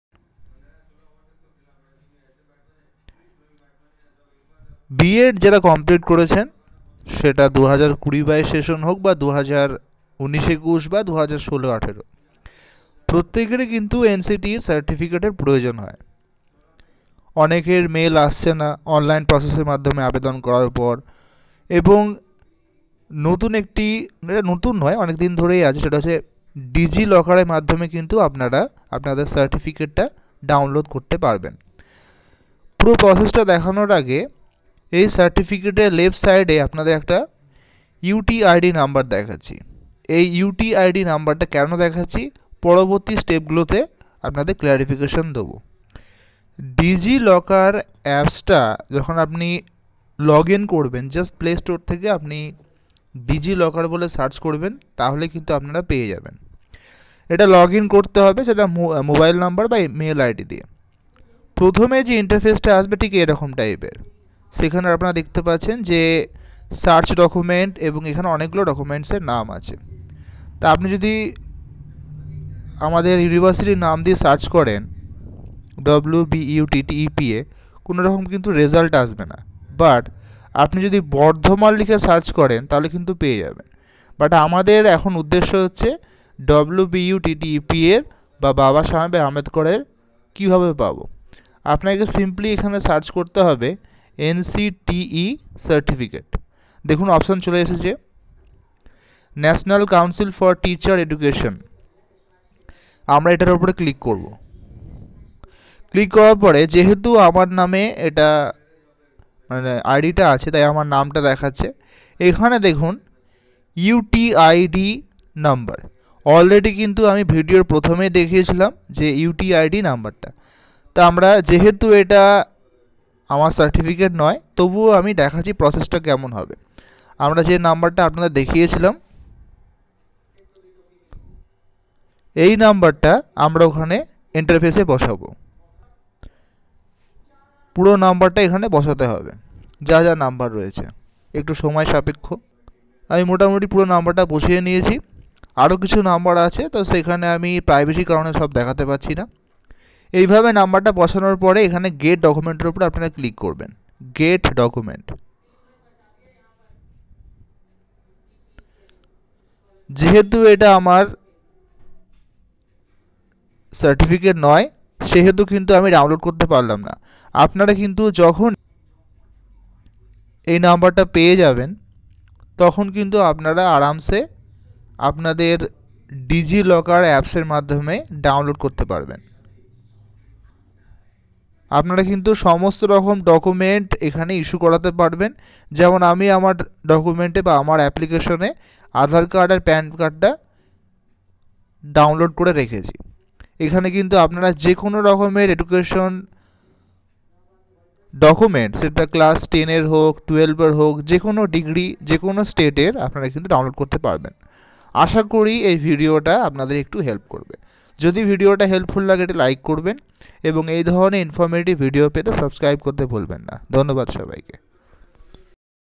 DOWNLOAD B.ED CERTIFICATE FROM DIGI LOCKER / DIGI LOCKER DOCUMENTS DOWNLOAD
Hi

GOOD NEWS
B.ED CERTIFICATE FROM
DIGITAL LOCKER

Instagram id
jamessannydey3